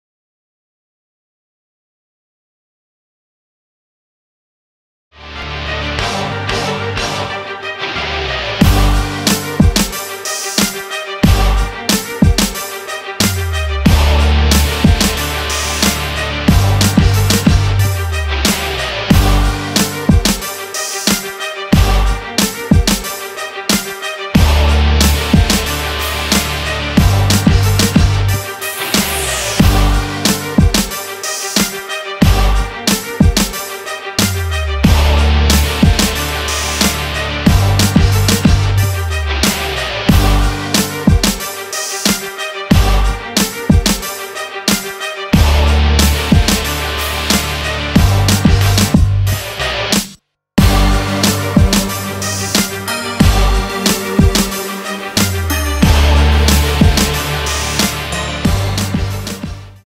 Vatche Papirian 2016 EBC West Mixtape - Class of 2022 Basketball
Vatche Papirian is a 7th grader from Los Angeles who had a big impact on the defensive end at the 2016 EBC West Camp.  The Class of 2022 prospect played the passing lanes and came up with steals throughout the camp.  Papirian made some very nice moves with the ball in his hands and finished the ball well at the rim.